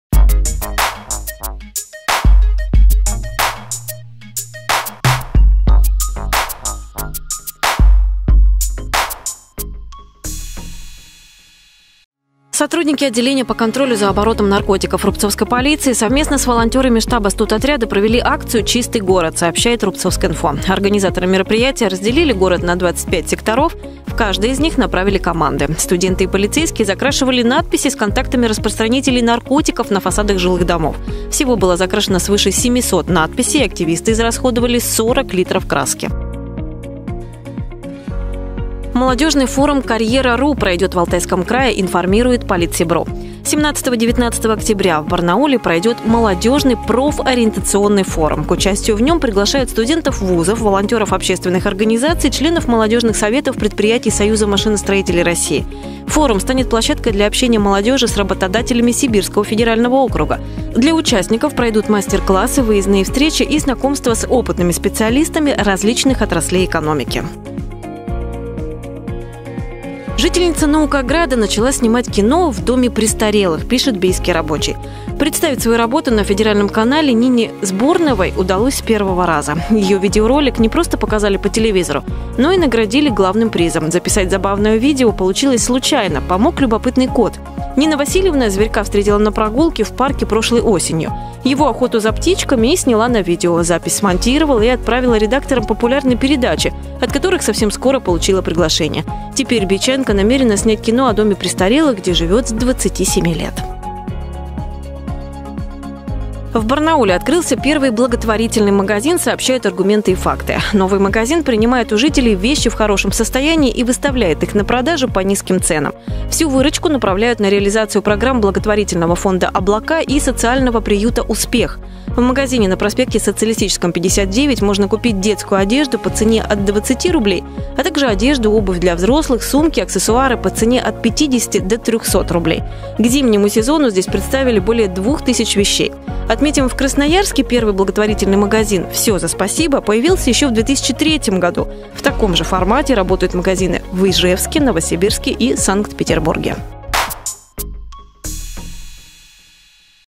О чем пишут издания «Сибирской медиагруппы» — обзор
В Рубцовске активисты закрасили более 700 надписей с рекламой наркотиков, а в Барнауле открылся первый благотворительный магазин. Подробнее об этом и не только — в обзоре новостей от Сибирской меадиагруппы.